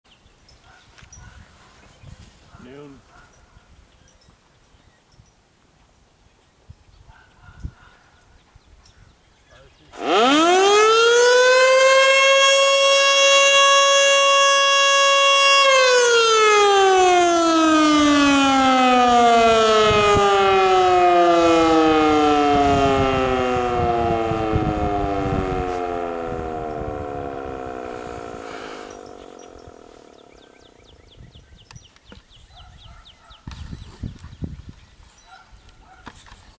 Federal Signal Model 2 Noon Whistle, Anchor, IL
Noon Whistle Meetup with TommyBNSF, Matteng1997, and American Siren Productions